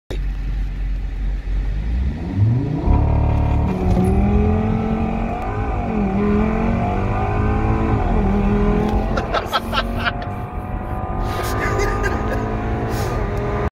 Audi RS3 - Launch Control 4500 rpm
RS3 Launch Control and RS3 Acceleration

Vehicle: 2016 Audi 8V1 RS3 (CZGB Engine) - This is the Pre Facelift RS3.

Current modifications:
Avon Tuning Stage 2 ECU
Avon Tuning Stage 2 TCU
Rogue Performance Catless Downpipe
Leyo Motorsport CAI
Forge Motorsport Intercooler
Autotech HPFP
GFB DV+